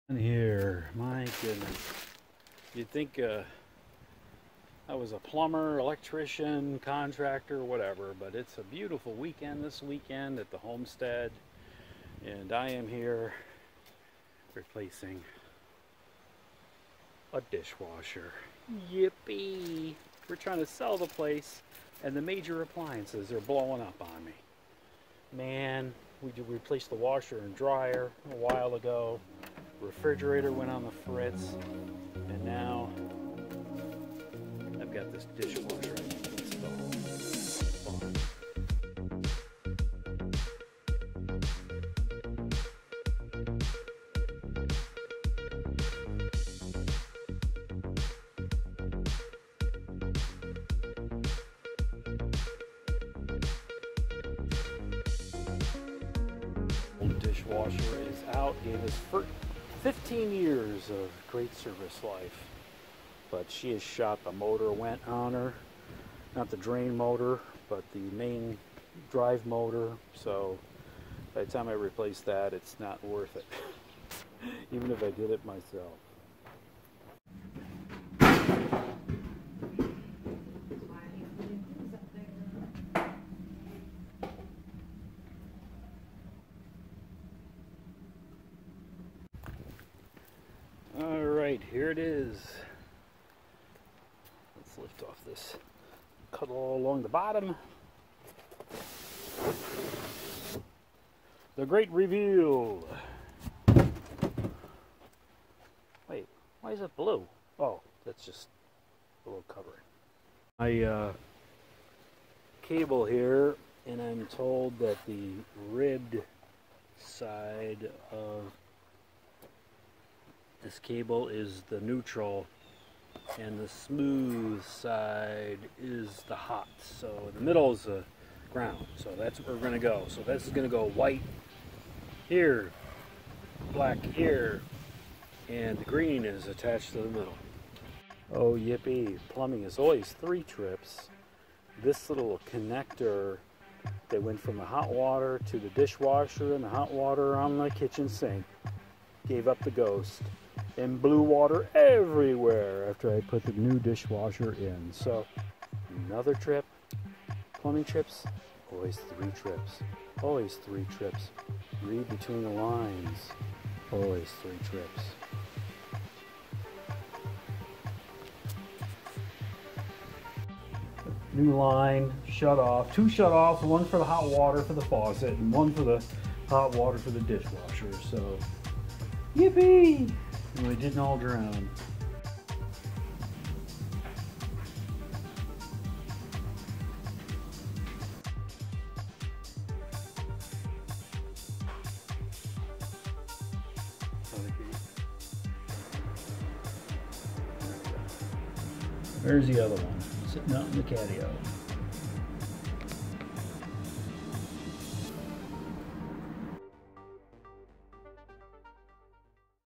Plumbingtakes3trips
Oh the fun I'm having not working on the RV. we're focused on getting the house ready to sell but the Old Gal just keeps tossing us back into focusing on her. This time it's the dishwasher. It was the main bath faucets, which we dealt with but since that was too easy why not toss in a major appliance? Not only that but call out any plumbing issue that has been OK for 15 years...